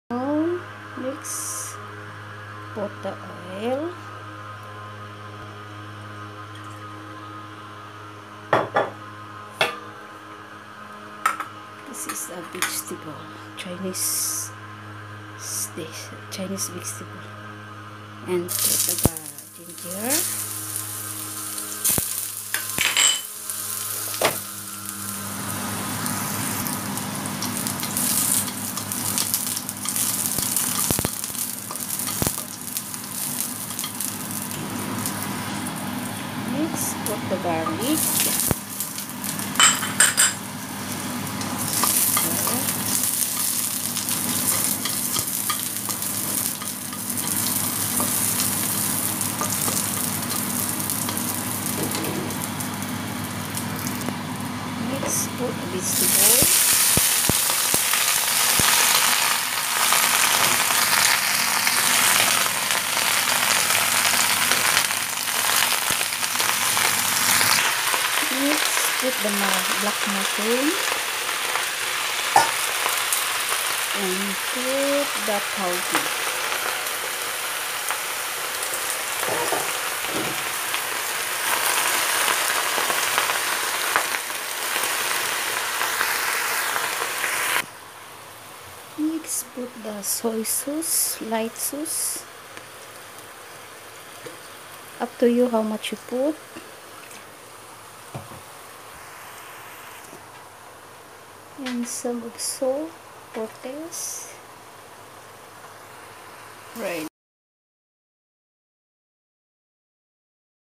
How to cook simple mixed vegetables, cabbage &black mushroom Lets start,guys/Cresilda's,Vlog
Hi guys!🙋‍♀️
welcome to my yt,channel
today i cook very simple vegetables!
here 👉🏾

(Stirfry,mixed vegetables)

ingredients:
long cabbage (wong bok)cut
black mushroom
beangurd,stick cut into shorter
ginger slice,,about 5 pieces
garlic,3-4 cloves
oil,salt.Ligth soy,sauce

bye bye💃stay safe always &Godbless us🙏